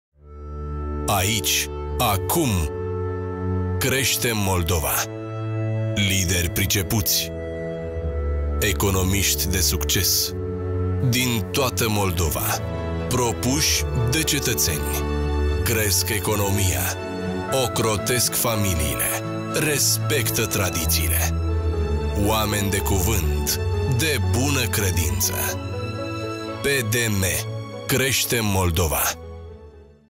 Spot electoral al PDM (nr.1) | Alegeri parlamentare 2014
Publicitate electorală a Partidului Democrat din Moldova difuzată la posturile TV din Republica Moldova în campania electorală pentru alegerile parlamentare din 30 noiembrie 2014 (spot nr.1)